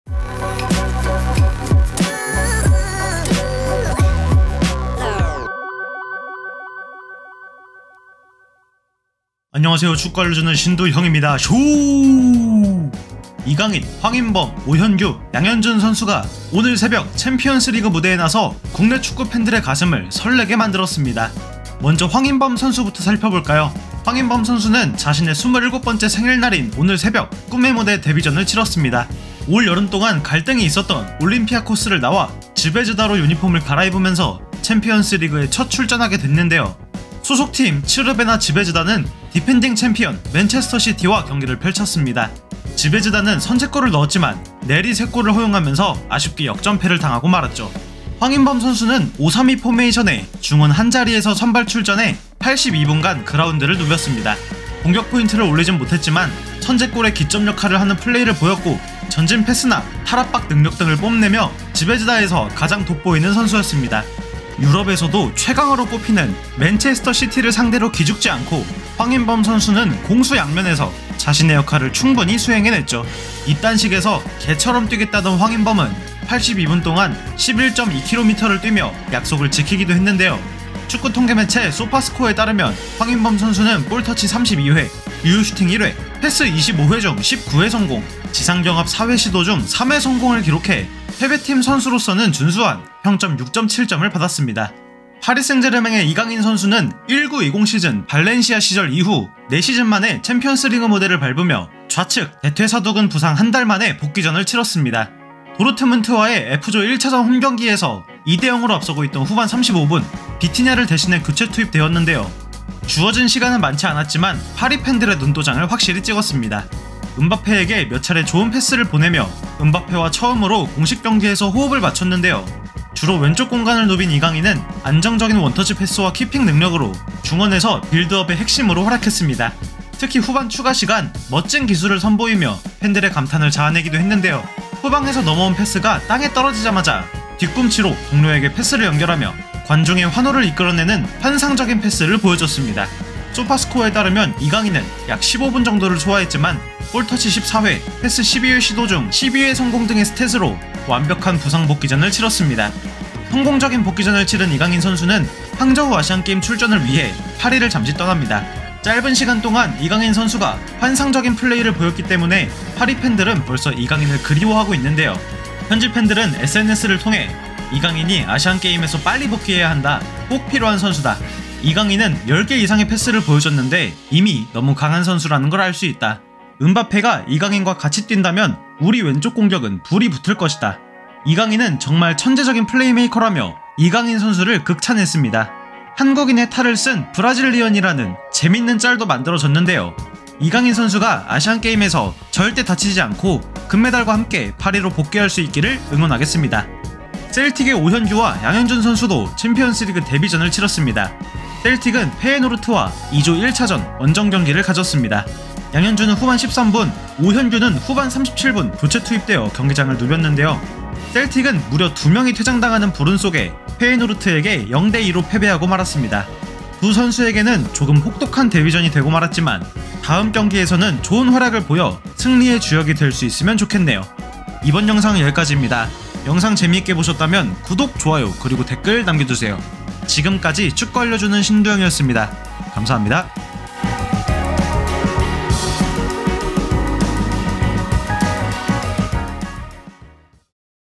10분 만에 존재감 드러낸 이강인, 생일날 챔스 데뷔한 황인범… 오늘 새벽 챔스 무대를 빛낸 한국 선수들 활약상은?
안녕하세요! 축구알려주는신두형입니다 Siu!
오늘 가져온 소식은 챔피언스리그 무대를 빛낸 코리안리거들의 소식입니다!
파리 생제르맹의 이강인, 츠르베나 즈베즈다의 황인범,
셀틱의 오현규와 양현준이 오늘 새벽 챔피언스리그 경기에 출전했는데요.

저 신두형은 사우디 GOAT가 되기 위해 항상 노력할게요!
감사합니다 Siu!